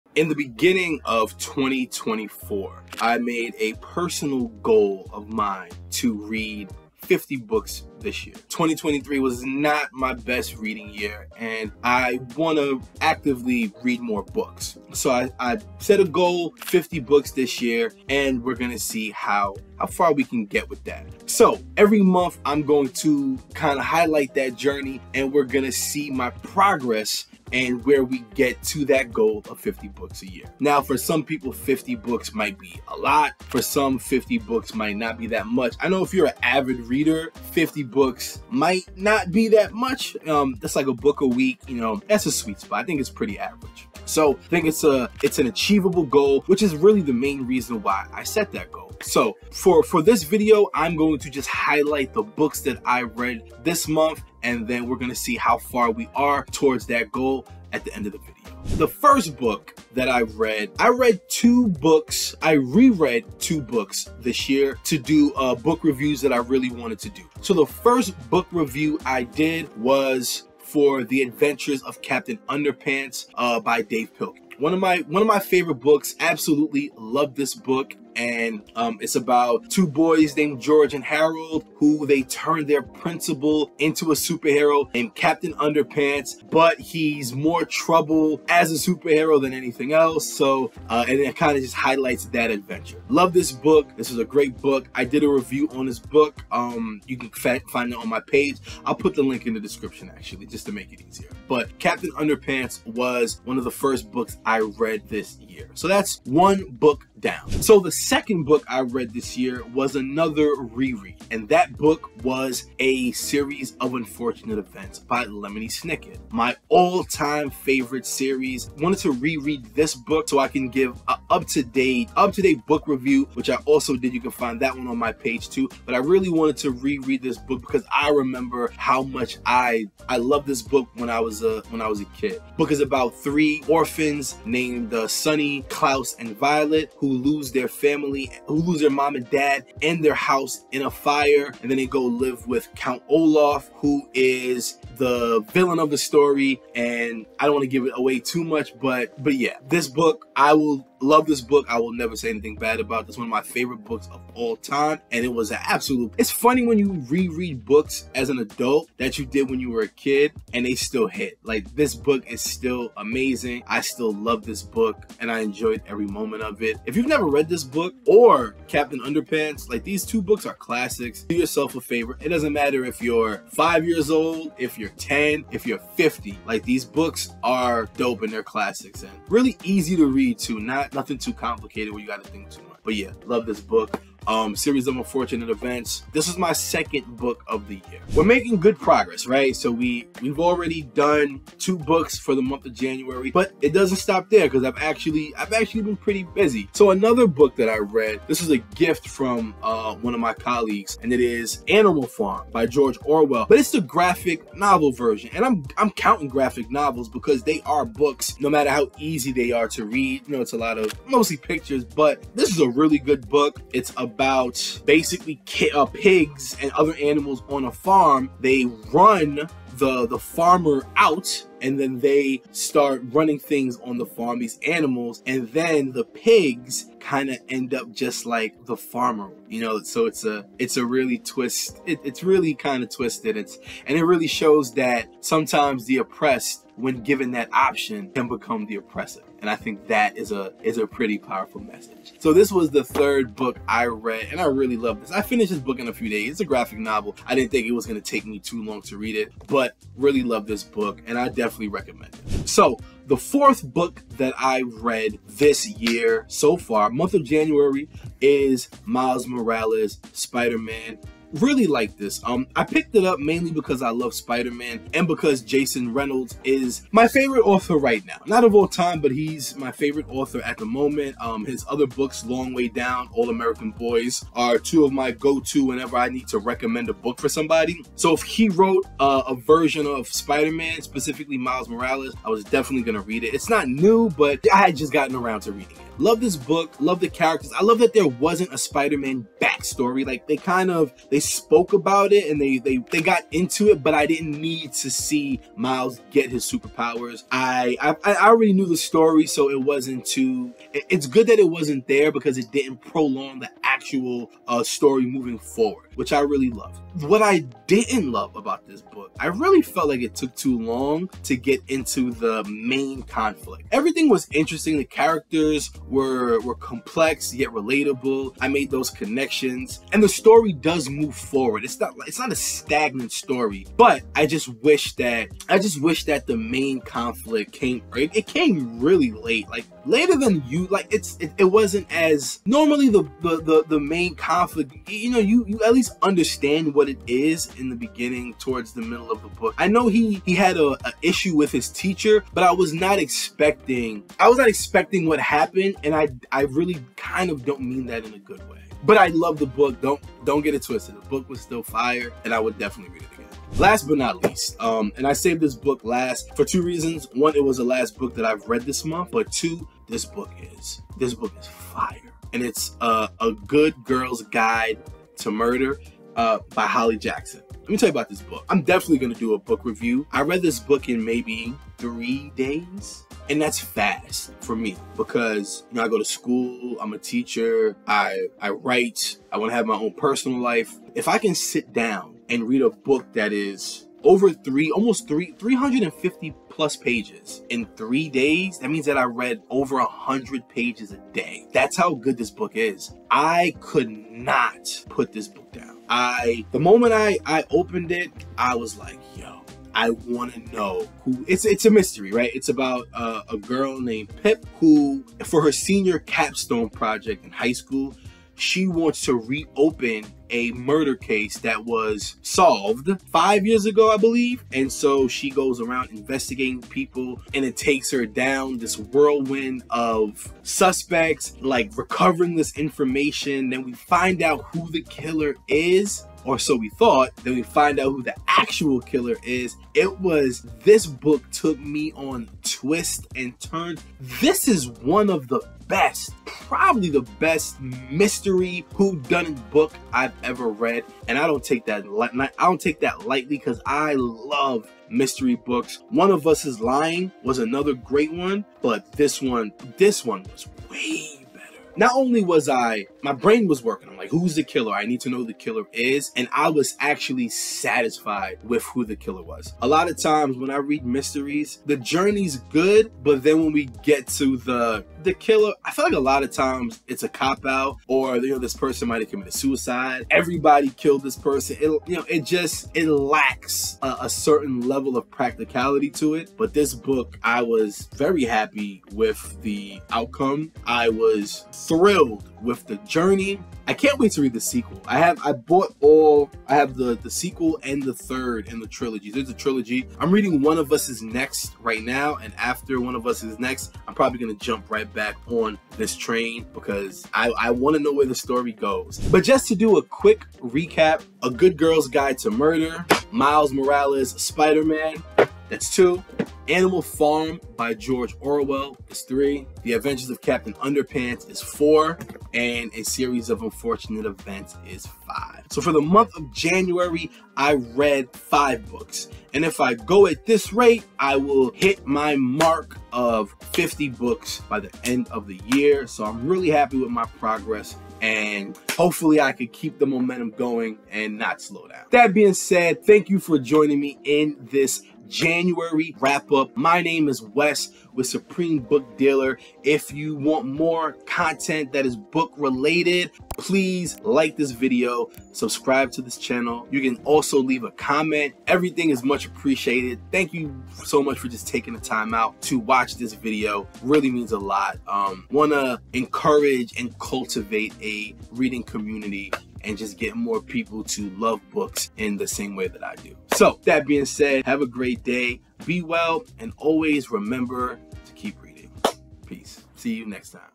January Wrap-Up! 5 Books: Mystery, Classics & More
In this video, I wrap up all the books I read in the month of January.